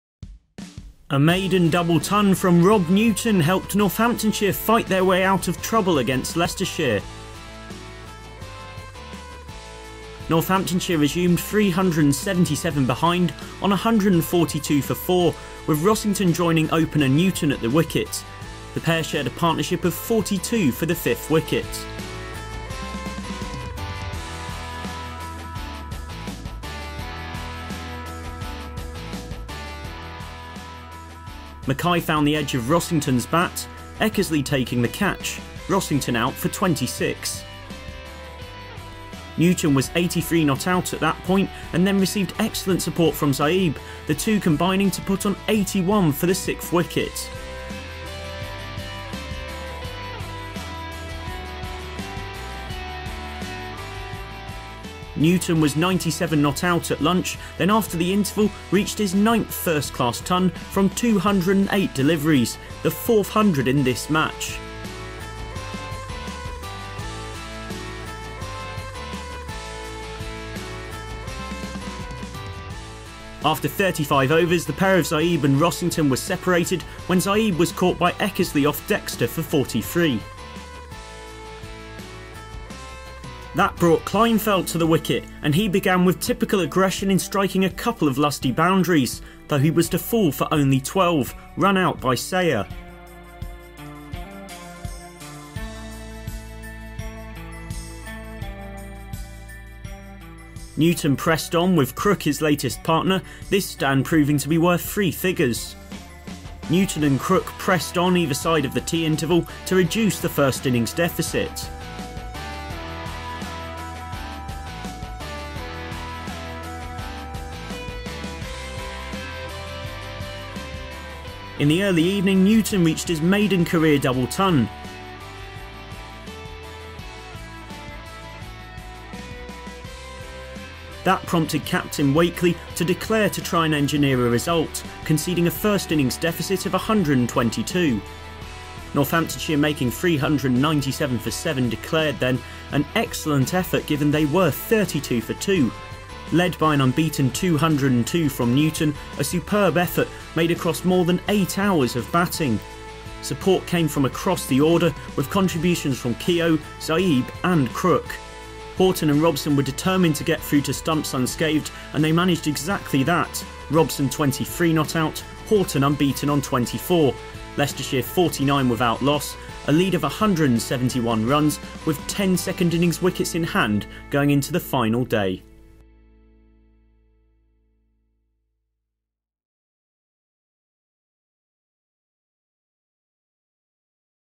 Specsavers County Championship: NCCC Vs Leicestershire: Day 3
Rob Newton’s maiden first-class double century led Northamptonshire out of danger on the third day at Wantage Road. Newton’s 202 not out led Northants to 397 for 7 against Leicestershire. They declared 122 behind to potentially help set up a final day run chase. Leicestershire closed on 49 for 0 in their second innings, leading by 171.